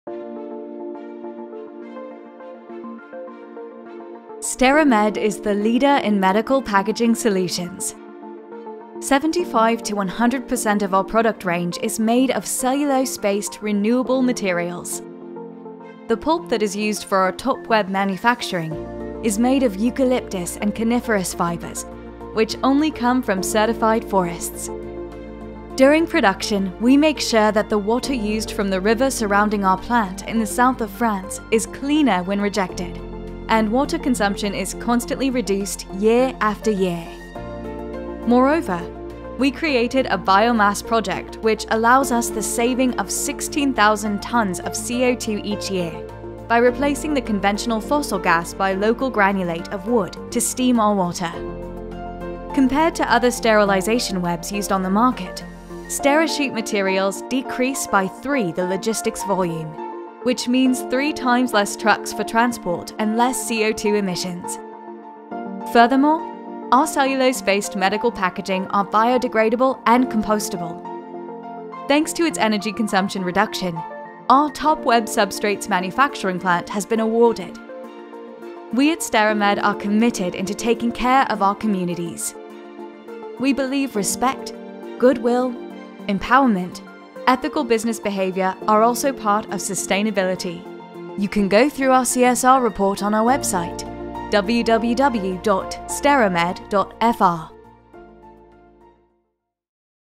STERIMED caring for environmental footprint
Discover how Sterimed Infection Control cares for its environment by actively reducing its footprint on a day to day basis.